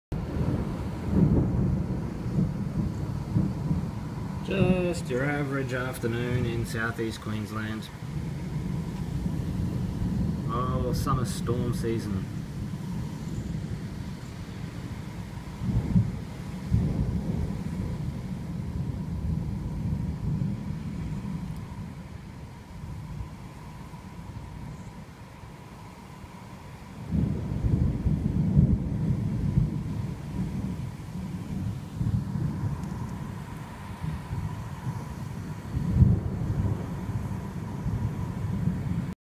Wild weather smashes south east
Yet another crazy intense storm full of  lightning thunder hail heavy rain and strong winds